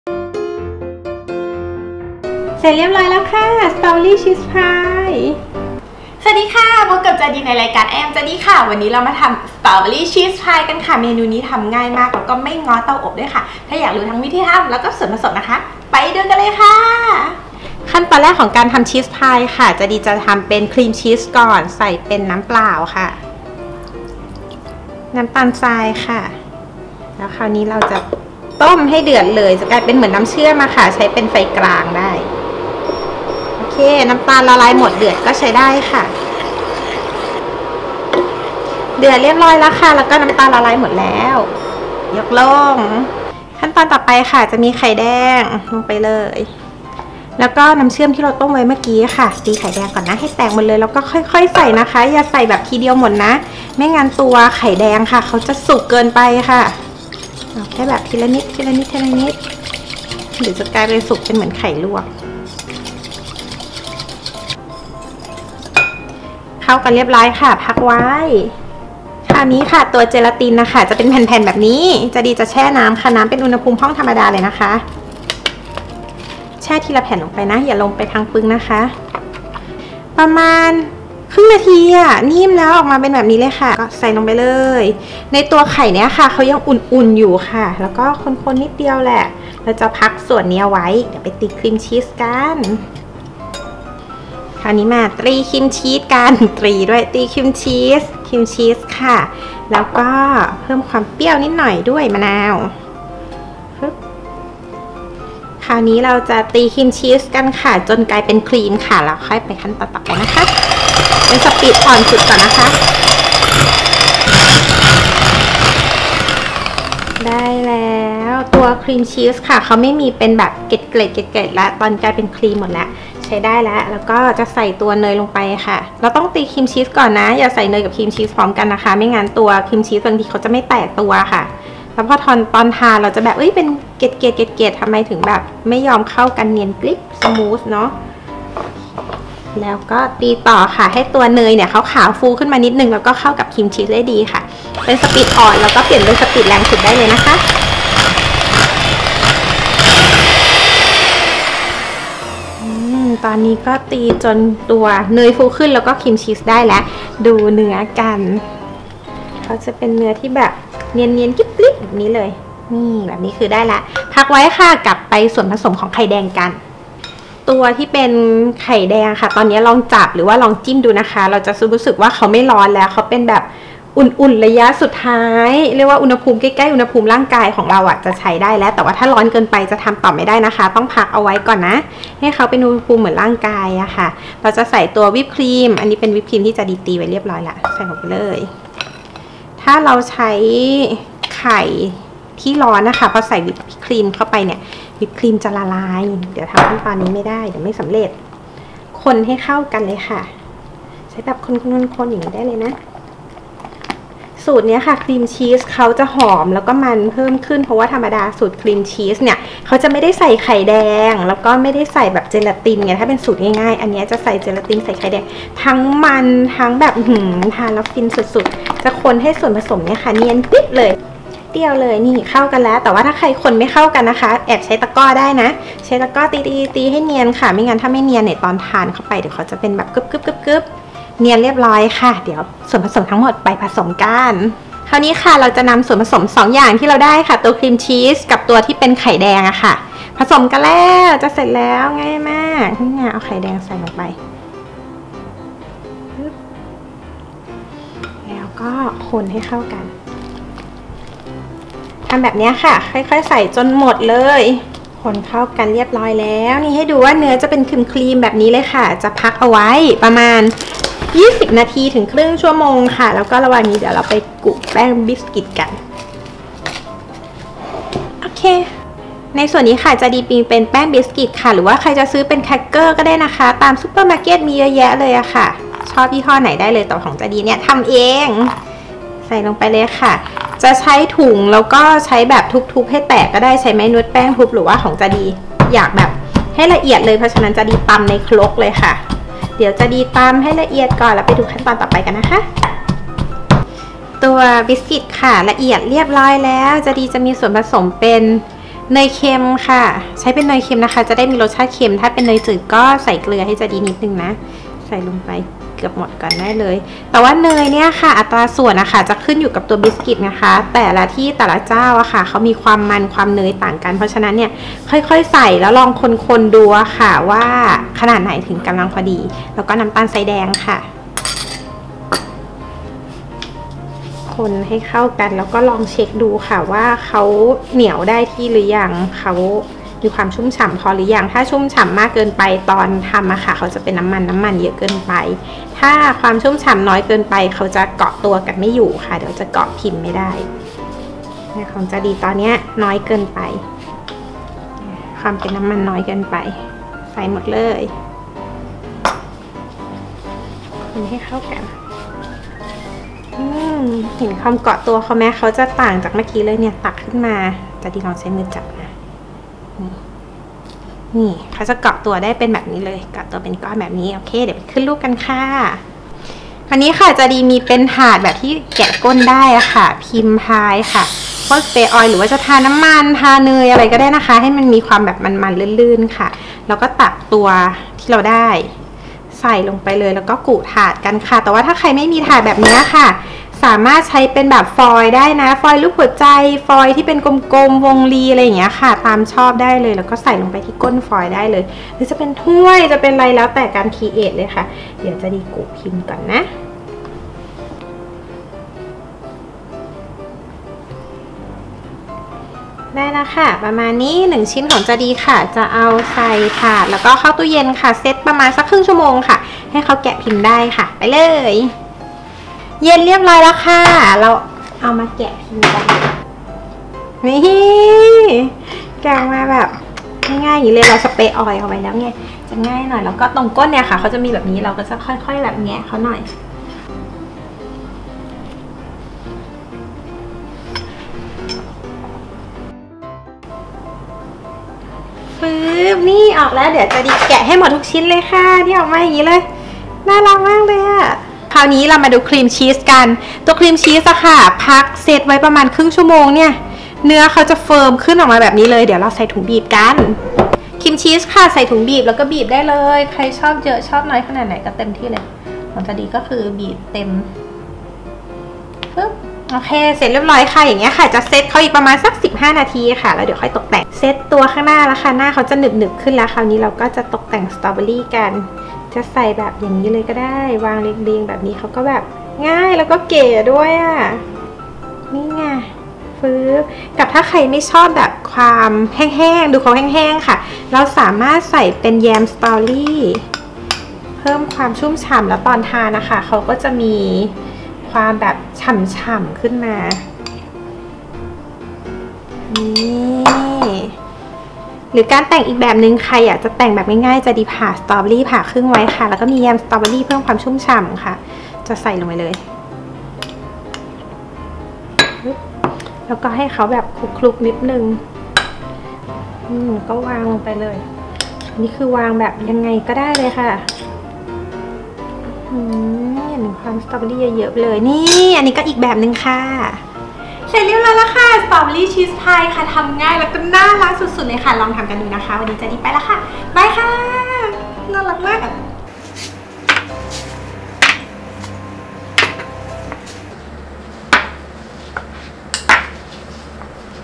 สตรอเบอร์รี่ชีสพาย(เชฟจาดี)(I am Jadee)
สตรอเบอร์รี่ชีสพาย

ส่วนผสมครีมชีส
1.น้ำตาลทราย 65 กรัม
2.น้ำเปล่า 62 มล.
3.ไข่แดง 3 ฟอง
4.เจลาติน 4 แผ่น
5.ครีมชีส 125 กรัม
6.น้ำมะนาว 1 ลูก
7.เนยเค็ม 125 กรัม
8.วิปครีม 200 กรัม

ส่วนแป้งพาย
1.บิสกิต 250 กรัม
2.เนยเค็ม 80 กรัม
3.น้ำตาลทรายแดง 1 ชต

ใครลองทำแล้วเป็นยังไงบ้าง ไปพุดคุยกันในเพจเฟสบุ๊คกันค่ะ